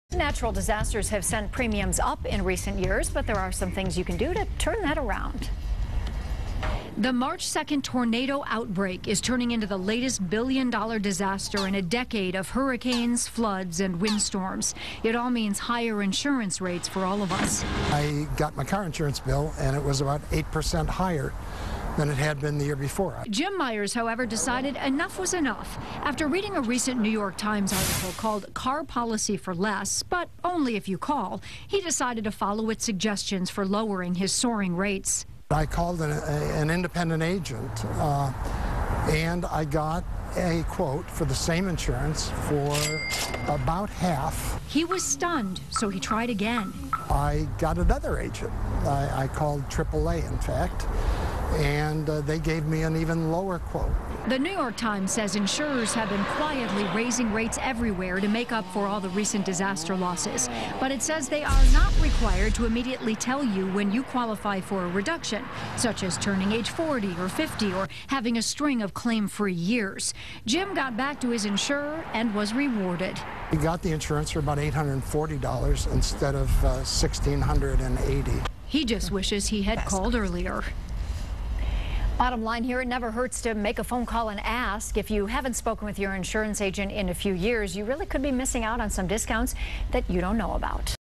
Fighting insurance rate hikes
Here are some tips to fight insurance rate hikes.